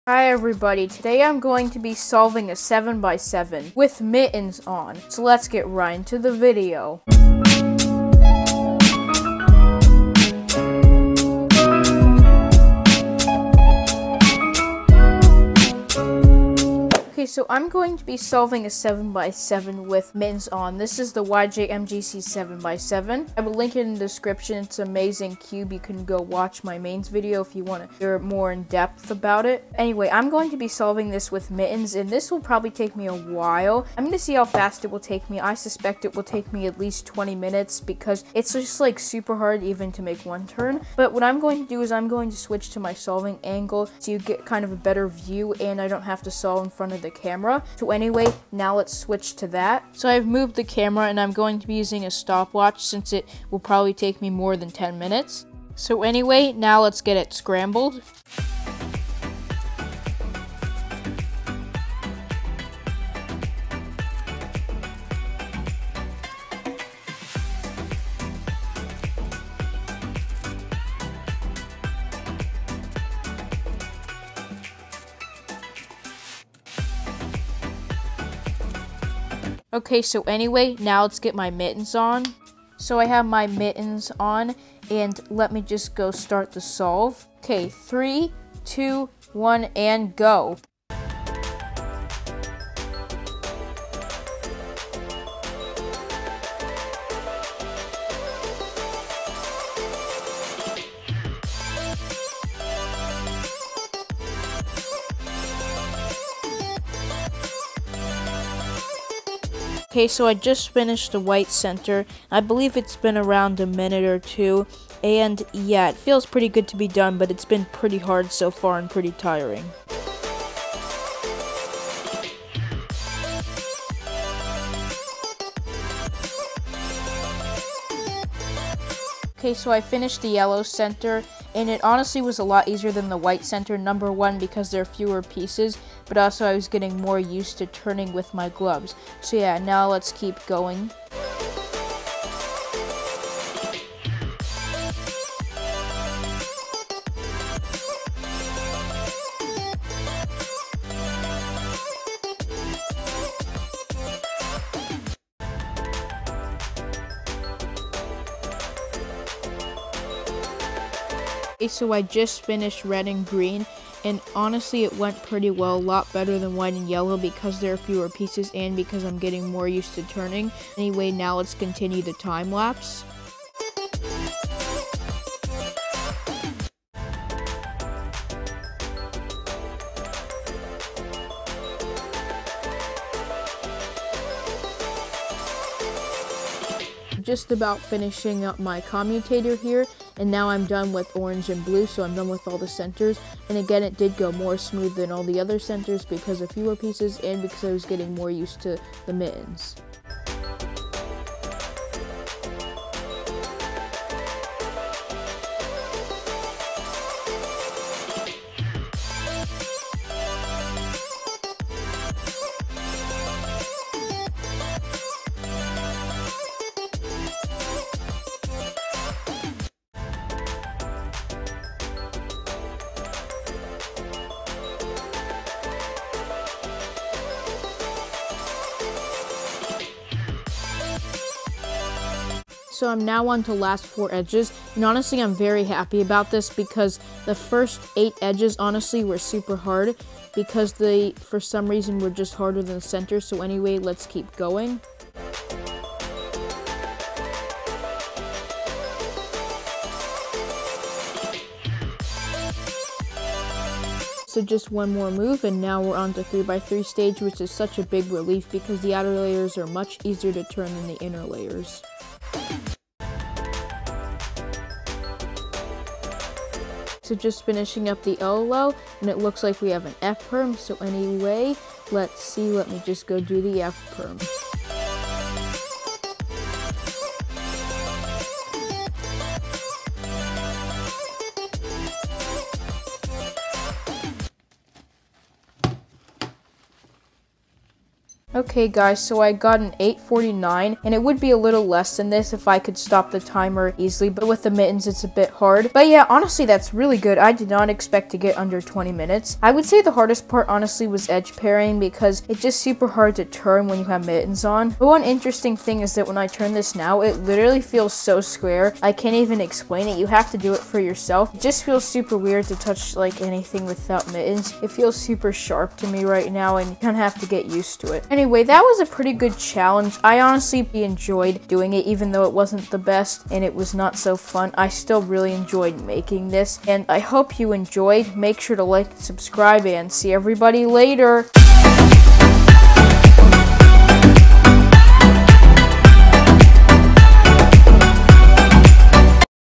SOLVING A 7X7 WITH MITTENS (HOW FAST CAN I DO IT?)
Today I will be doing a very hard challenge, solving a 7x7 with mittens! This was very hard and I would definitely not recommend you do it. How long do you think it took? And also please comment if you like these type of challenge videos.
Please also comment any video ideas you would like to see.